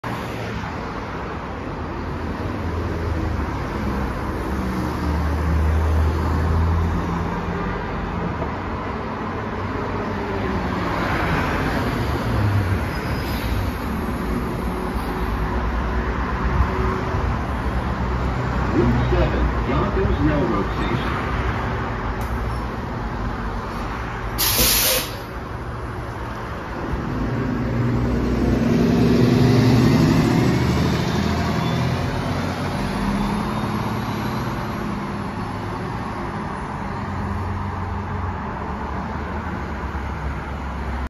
Bee-Line Bus New Flyer XDE40 Route 7 To YONKERS RR STATION #477 YONKERS AVE ALL ELECTRIC HYBRID BUS
FLEET 2022 HEV